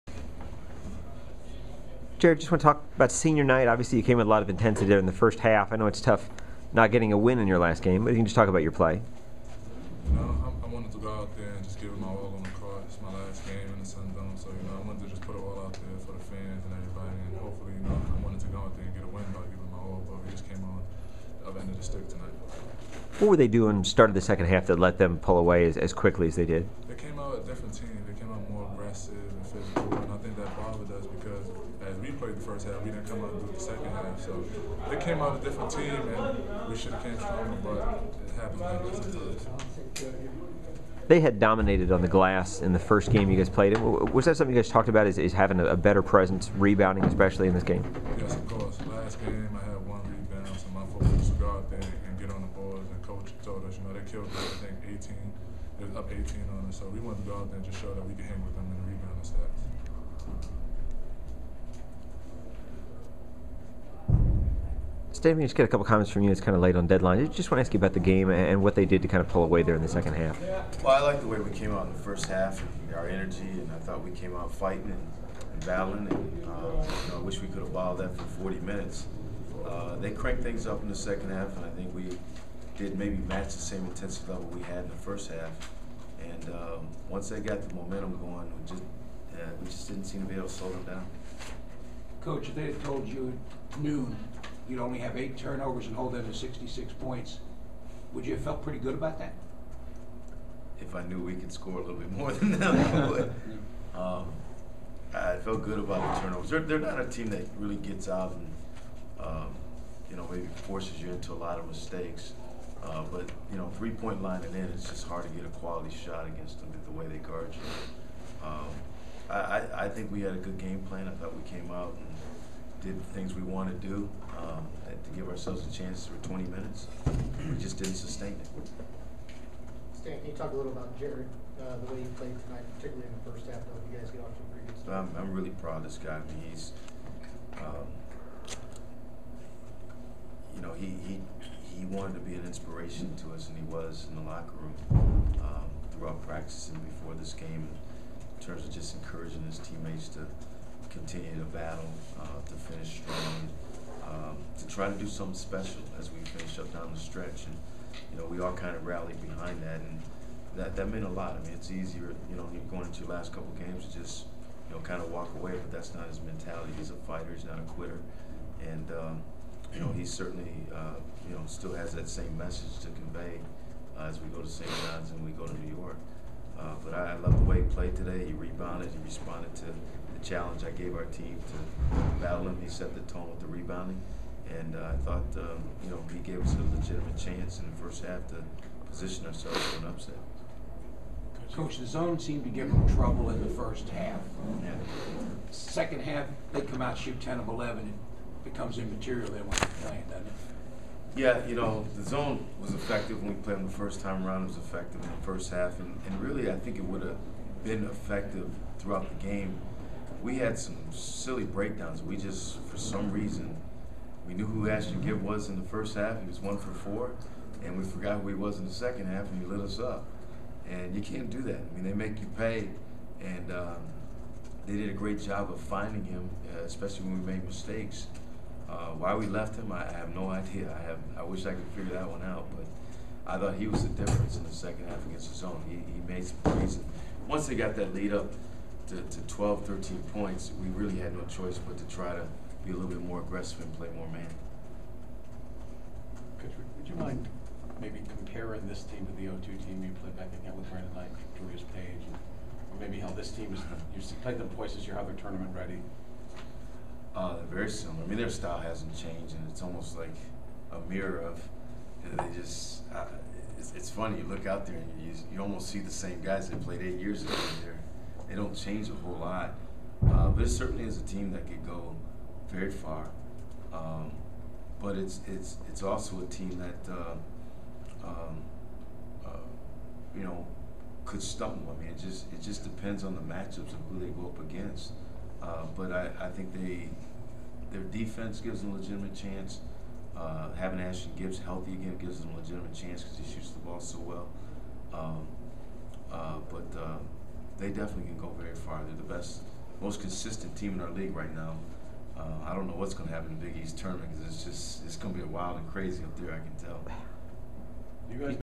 USF's Jarrid Famous and Stan Heath after loss to Pittsburgh
USF senior Jarrid Famous and head coach Stan Heath talk after the Bulls' 66-50 loss to Pittsburgh in their final home game of the 2010-11 season.